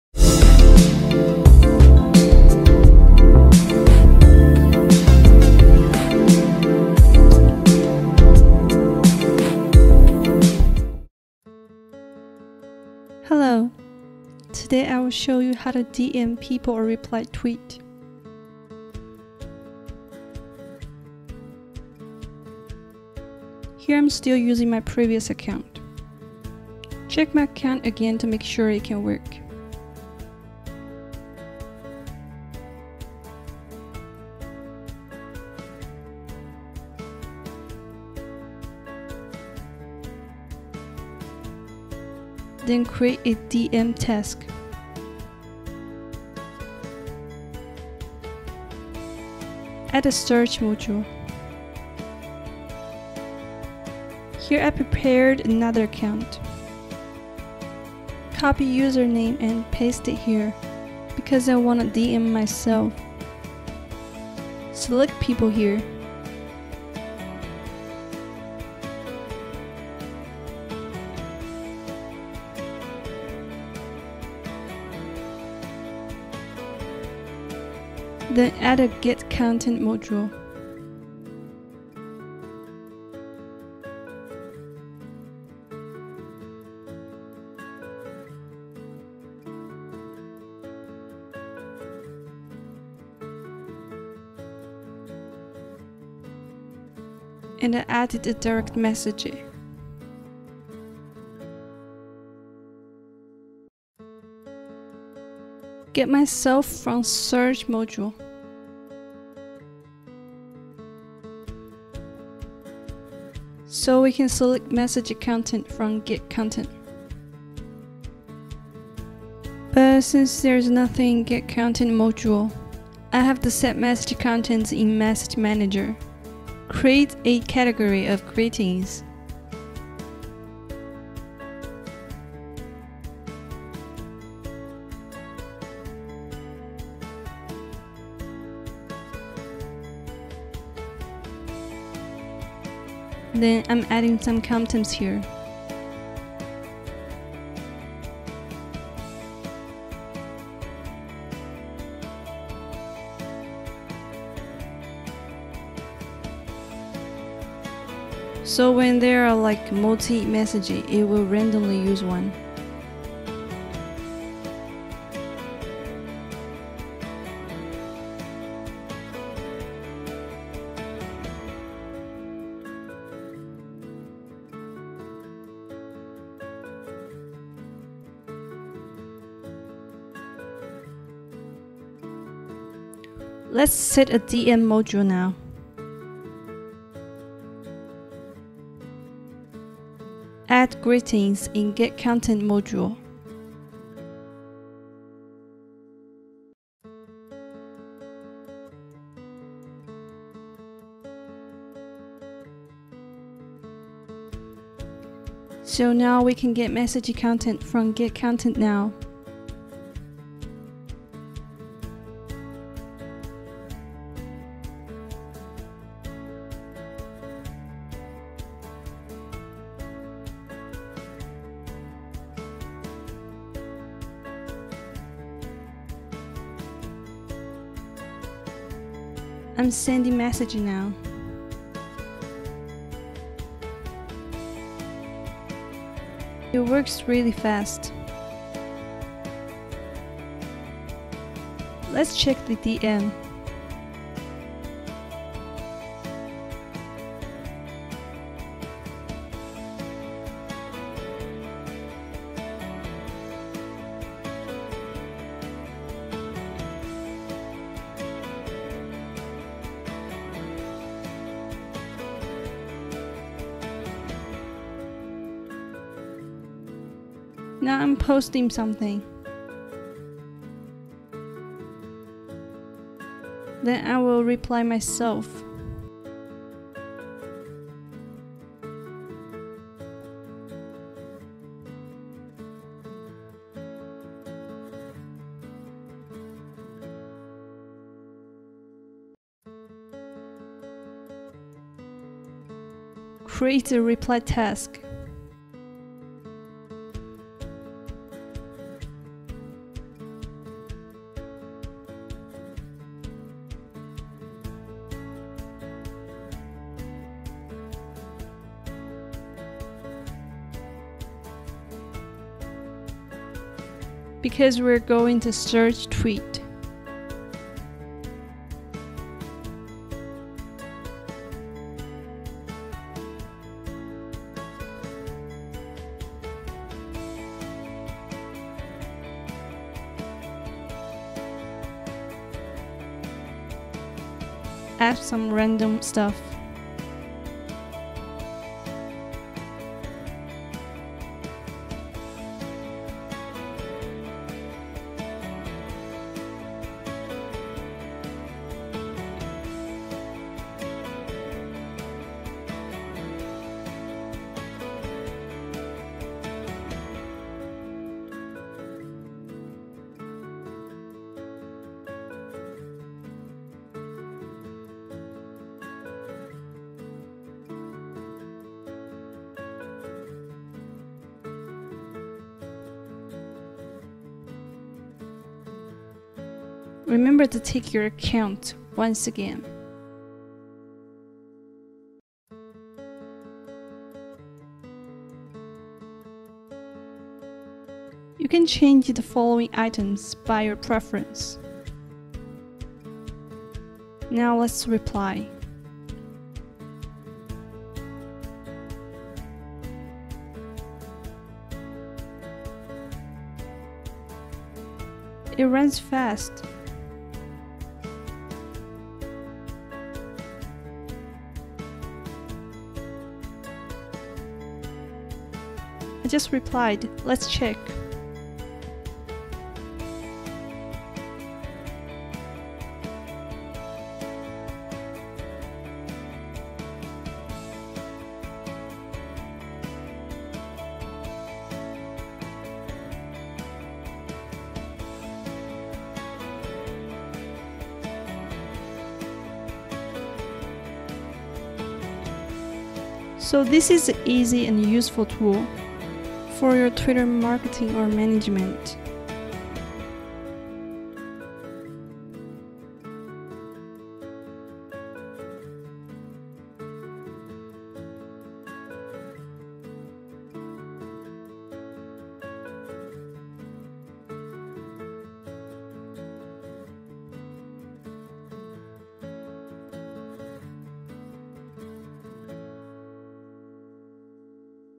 Twitter automation, auto like, follow, comment, follow, retweet|TweetAttacksPro 4 Tutorial Part 2
TweetAttacksPro is developed by WhiteHatBox.
It is functional and easy to use.
This video is to show you how to use it.

Auto-follow, like,commet, reply, retweet, send DM/direct message on Twitter